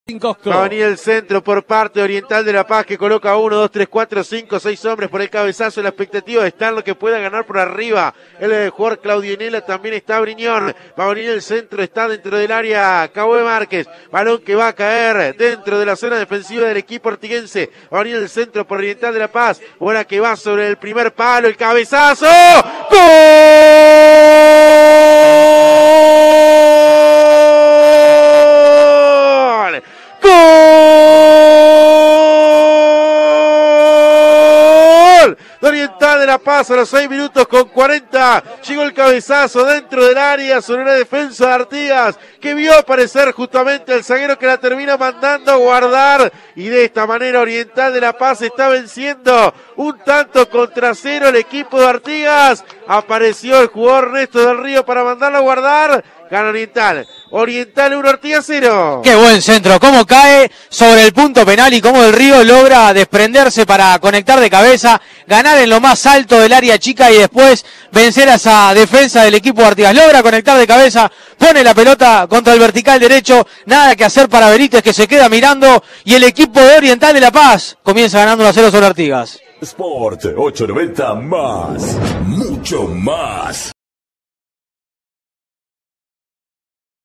Oriental de La Paz 1 Artigas 0 - Primera C 2022 (por Nicolás Pirri)
El gol del partido, por Nicolás Pirri y Mauricio Raggiotto para Sport 890 Más.